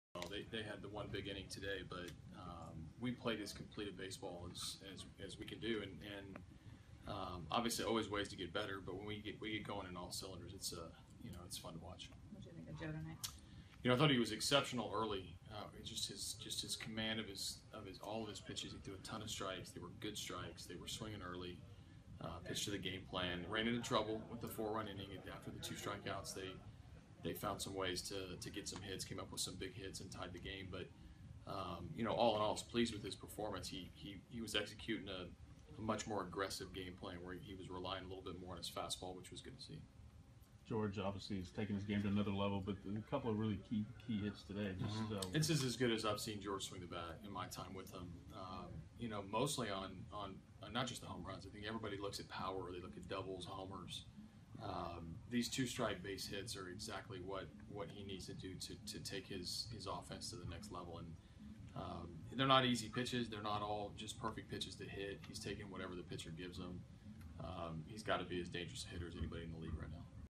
HOU@ATL: Hinch on clinching two-game set vs. Braves
Astros manager A.J. Hinch talks about his team knocking seven doubles for the third straight night and Joe Musgrove's solid outing